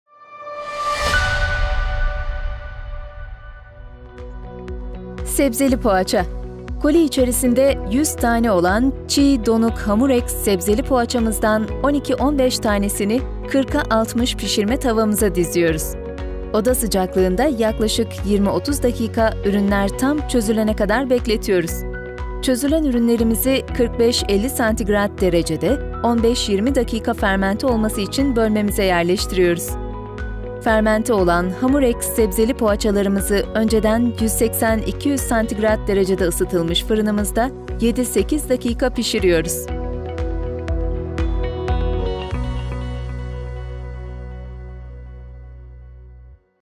SEBZELİ POĞAÇA
Koli içerisinde 100 tane olan çiğ donuk Hamurex sebzeli poğaçamızdan 12 -15 tanesini 40x60 pişirme tavamıza diziyoruz.

Oda sıcaklığında yaklaşık 20-30dk  ürünler tam çözülene kadar bekletiyoruz.

Çözülen ürünlerimizi 45-50 derecede 15-20 dakika  fermente olması için bölmemize yerleştiriyoruz.

Fermente olan Hamurex sebzeli poğaçalarımızı önceden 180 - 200 derecede ısıtılmış fırınımızda 7-8 dakika pişiyoruz.